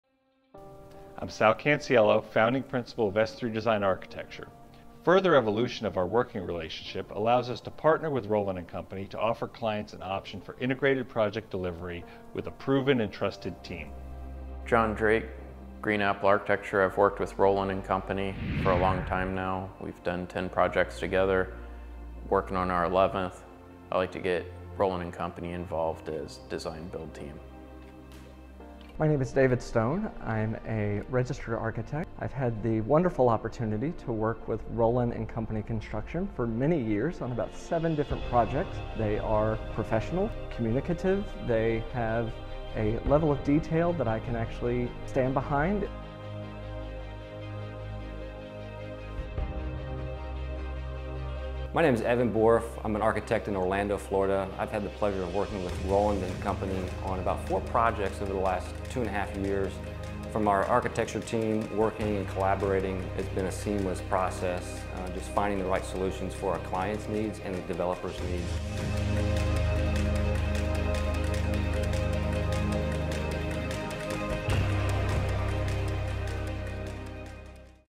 Rowland and Co "Video Card Edit"
Showcasing Rowland and Co's Commercial skills, featuring Architects from all over the nation.